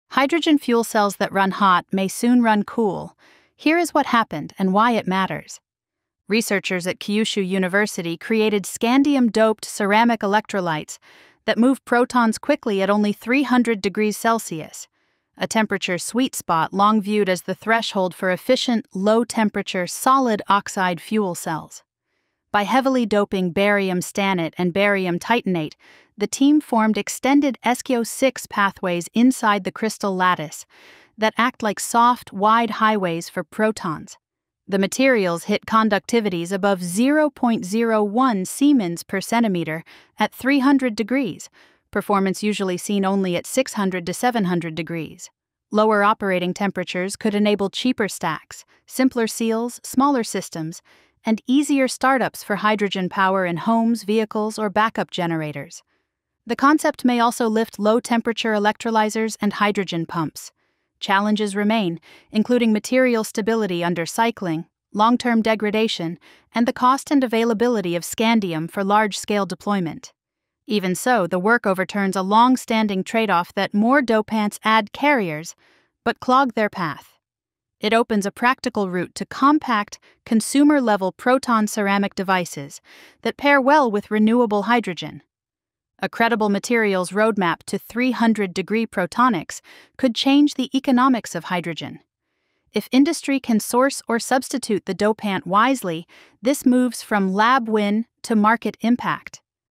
Cooler Hydrogen Fuel Cells
Scandium-doped ceramics achieved fast proton conduction at 300°C, promising cheaper, simpler hydrogen fuel cells and compact energy systems
ScienceDaily / Low-temperature protonics materials